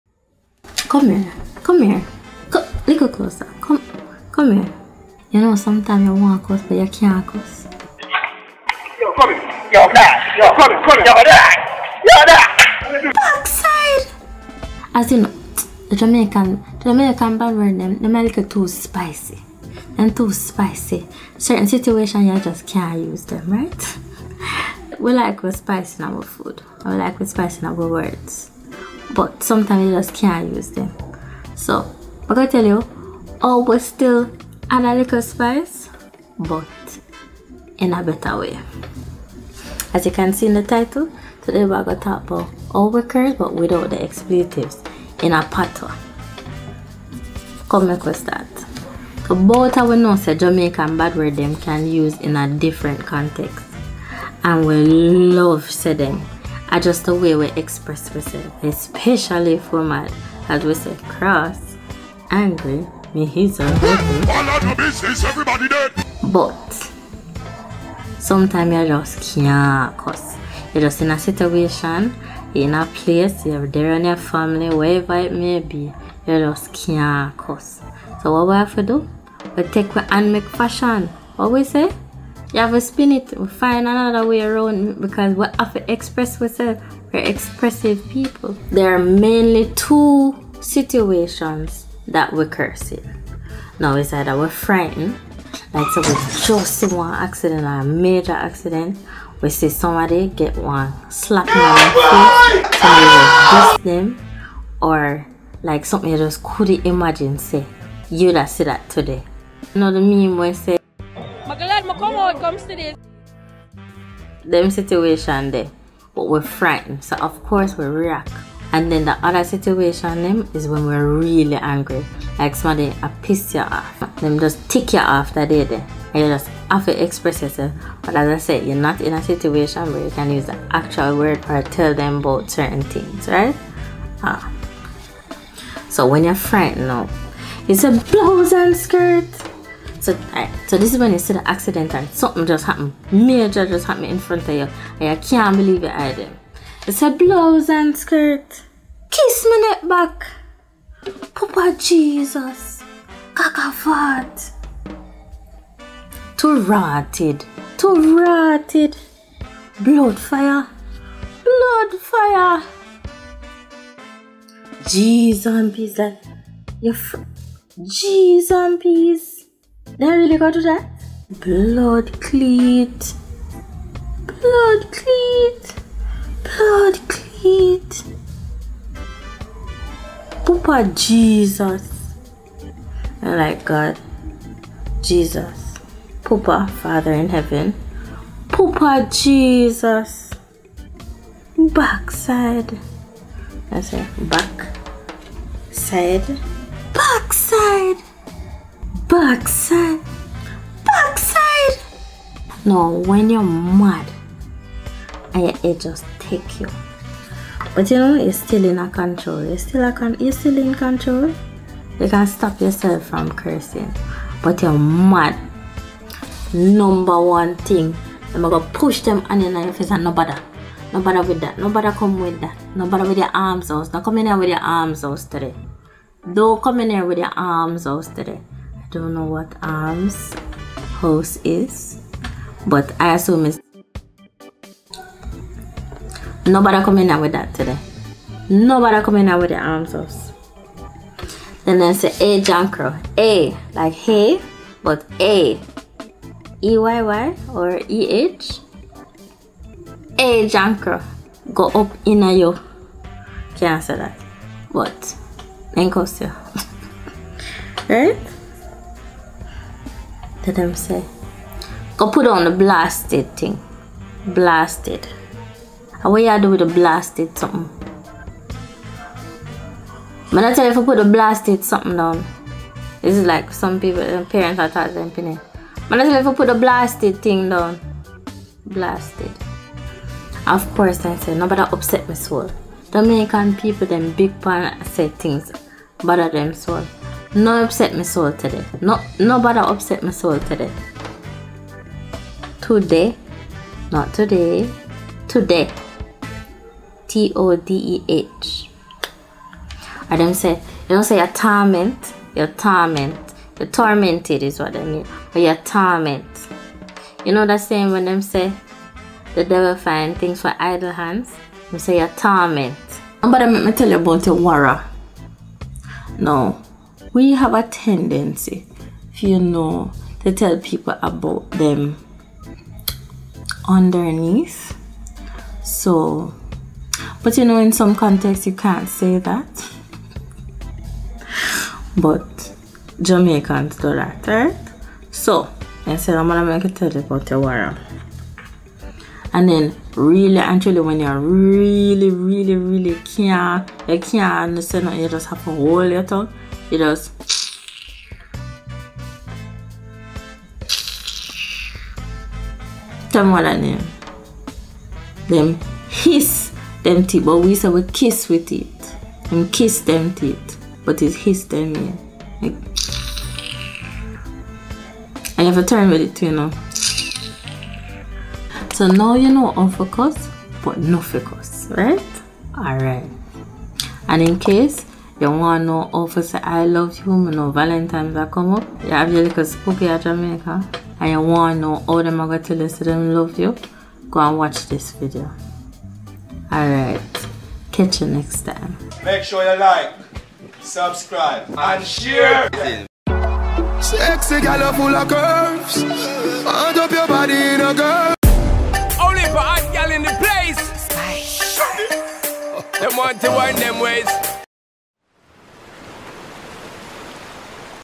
The "Clean Version" to JAMAICAN CURSE WORDS IN PATWA (Patois) | MCKNZ
In this hilarious and eye-opening video, we present the "Clean Version" of Jamaican curse words in Patwa (Patois). Join MCKNZ as we break down some of the most colorful expressions in Jamaican culture, translating them into fun, family-friendly terms!

Jamaican patois, especially our curse words are very colorful to say the least... and in some situations you can't curse, so we tun we hand mek fashion. Here are 20 ways we cuss yuh but without the expletives.

🌟 What to Expect:

A light-hearted exploration of Jamaican slang
Cultural insights into the meaning behind these phrases
A few laughs as we navigate the world of Patwa with a twist!
Whether you're familiar with Jamaican culture or just curious about the language, this video is for you!

Want a part 2? There are so many more we can add!

Keep di link!
IG: mcknz02 | Twitter: @mcknz02 | Tiktok: mcknz02

0:00 Intro
0:50 Context bad worse are used
1:30 Why we curse
2:18 "Clean Curse Words" use when frightened
3:30 Phrases used when angry